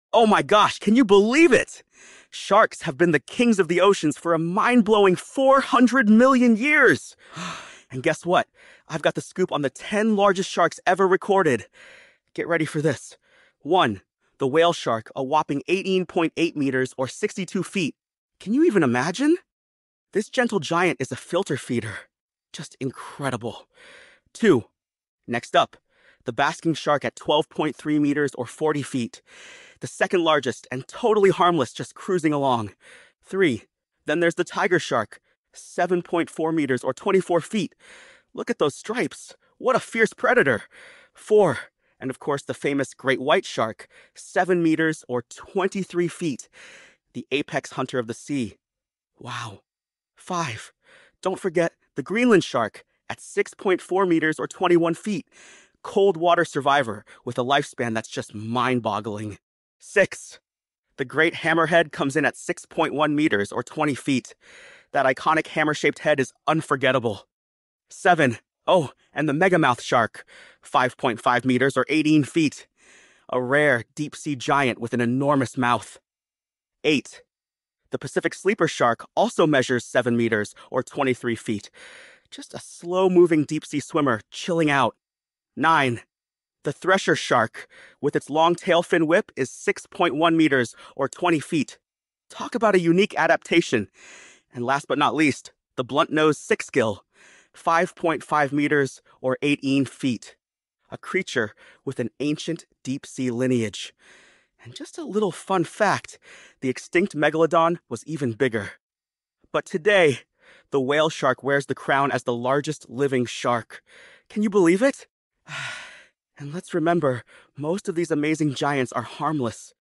Secret Series: Top 10 Largest Sharks Ever Recorded 🦈
🦈 Dive into the Secret Series as we reveal the 10 largest sharks ever recorded! From the gentle Whale Shark to the feared Great White, these ocean giants showcase the incredible diversity of shark species.

🌊 Highlights include:

Whale Shark – 18.8m / 62ft – the largest living shark

Basking Shark – 12.3m / 40ft – harmless filter feeder

Tiger Shark – 7.4m / 24ft – striped predator

Great White Shark – 7m / 23ft – apex hunter

Greenland Shark – 6.4m / 21ft – centuries‑long lifespan

Great Hammerhead – 6.1m / 20ft – iconic head shape

Megamouth, Sleeper, Thresher & Sixgill – rare deep‑sea giants

⚠️ Note: The extinct Megalodon was even bigger, but today the Whale Shark holds the crown as the largest living shark.